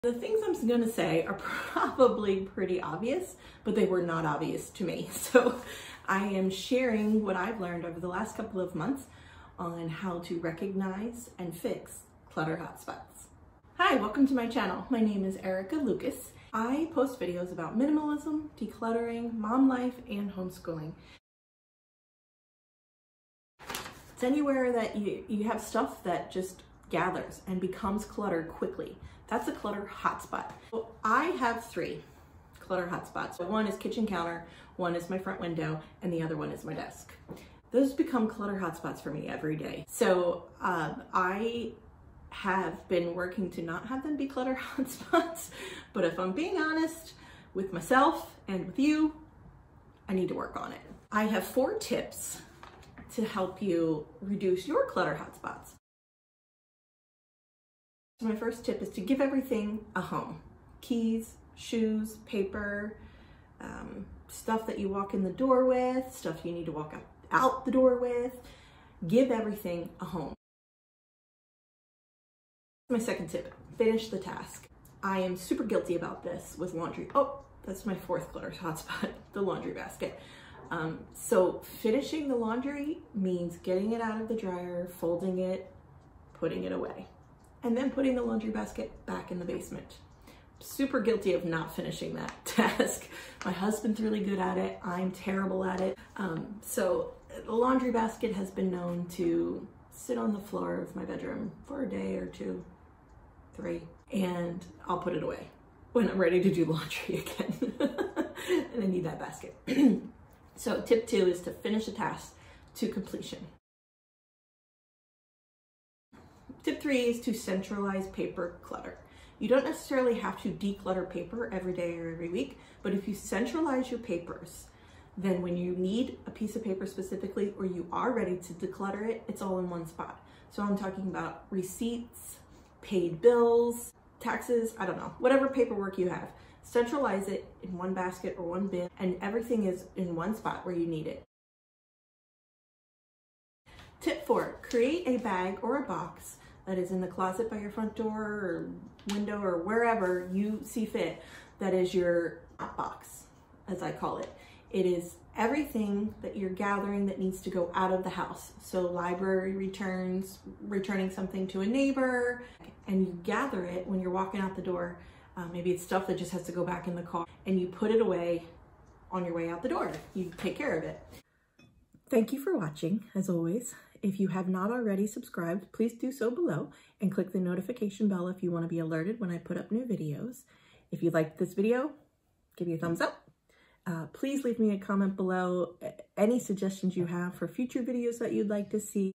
4 WAYS to MANAGE CLUTTER HOT SPOTS - Minimalism
What is a clutter hot spot? Anywhere you have stuff that just gathers and collects as clutter. Where in your house do you have clutter hot spots? Kitchen counter? Your desk? Watch this video for 4 tips to manage your clutter hot spots.

4 TIPS TO MANAGE CLUTTER HOT SPOTS - Minimalism - Manage the Daily Mess